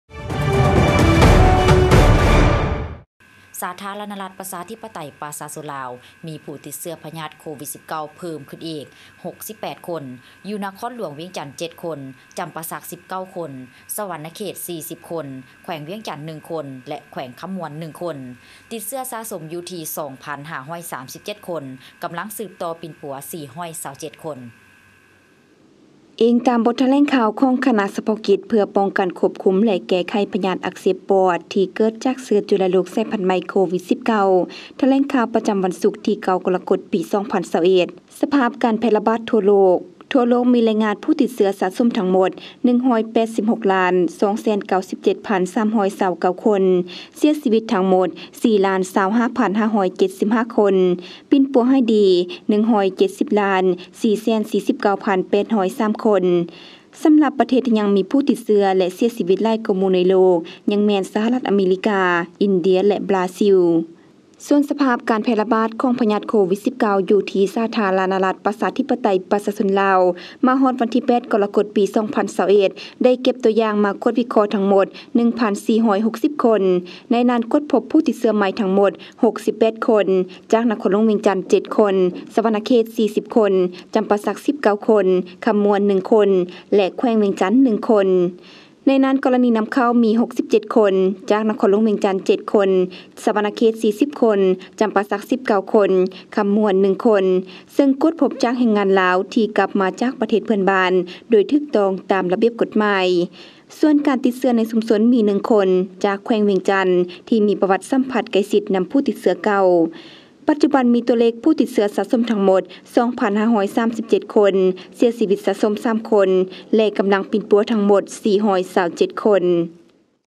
ວັນທີ 8 ກໍລະກົດ 2021 ສປປ ລາວ ມີຜູ້ຕິດເຊື້ອໂຄວິດ 19 ເພີ່ມ 68 ຄົນ
ສະຖານີ ໂທລະພາບ ປ້ອງກັນຄວາມສະຫງົບ  LAO PSTV12/07/2021
ວັນທີ 8 ກໍລະກົດ 2021 ສປປ ລາວ ມີຜູ້ຕິດເຊື້ອໂຄວິດ 19 ເພີ່ມ 68 ຄົນ, ນະຄອນຫຼວງວຽງຈັນ 7 ຄົນ, ຈຳປາສັກ 19 ຄົນ, ສະຫວັນນະເຂດ 40 ຄົນ, ແຂວງວຽງຈັນ 1 ຄົນ, ແຂວງຄຳມ່ວນ 1 ຄົນ ຕິດເຊື້ອສະສົມ 2.537 ຄົນ, ກຳລັງສືບຕໍ່ປິ່ນປົວ 427 ຄົນ